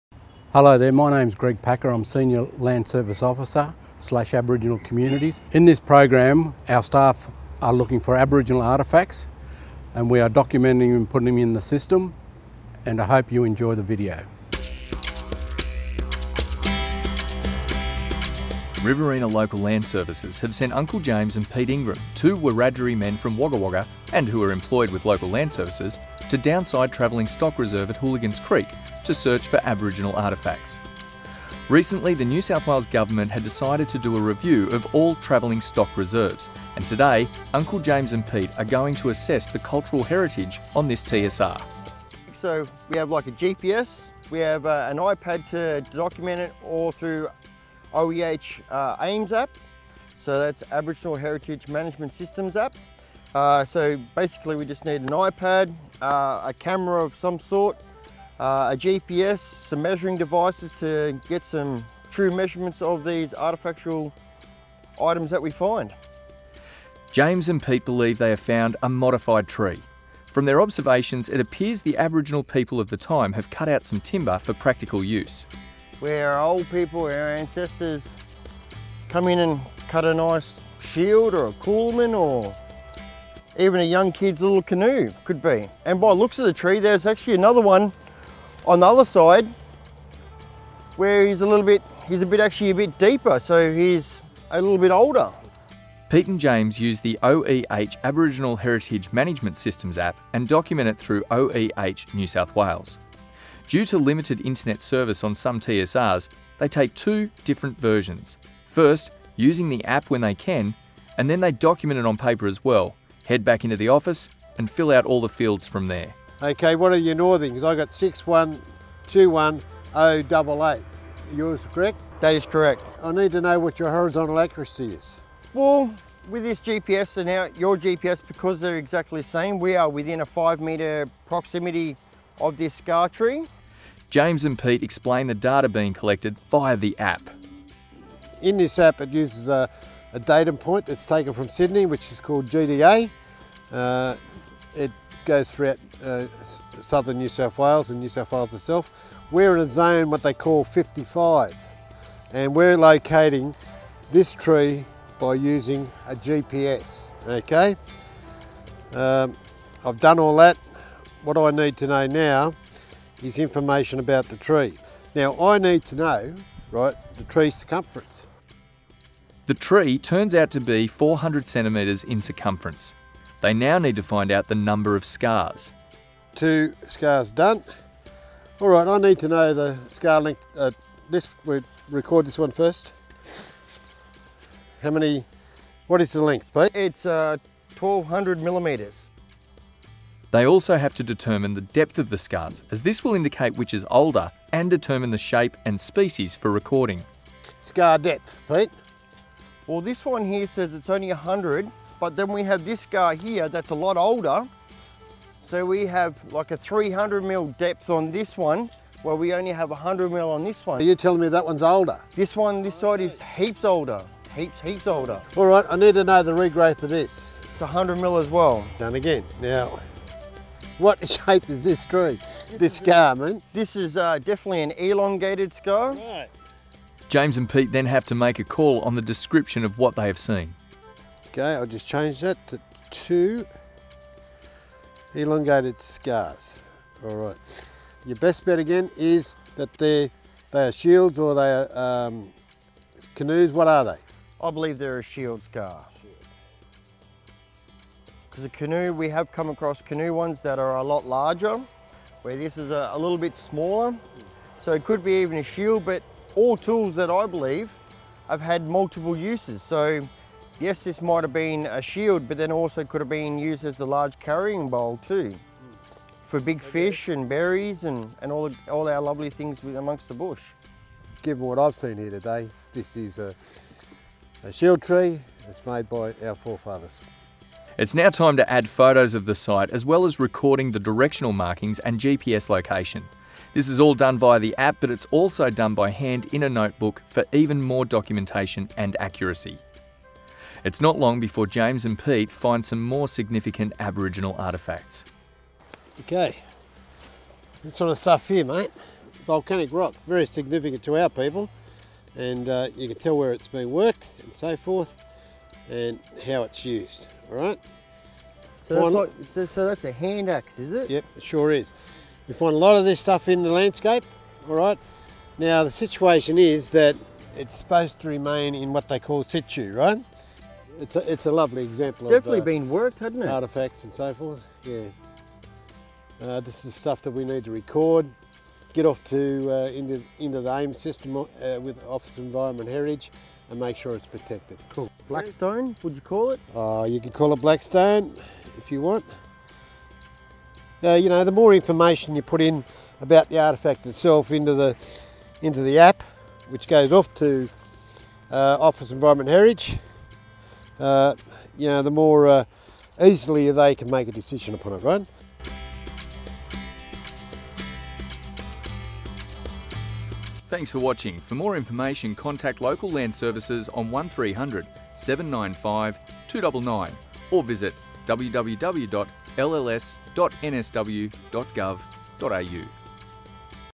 Protecting cultural values
Local Land Services Aboriginal Communities Officers James Ingram and Peter Ingram discover Aboriginal artefacts on travelling stock reserves with the Riverina region.  James and Pete talk through the documentation of the artefacts through the Office of Environment and Heritage app.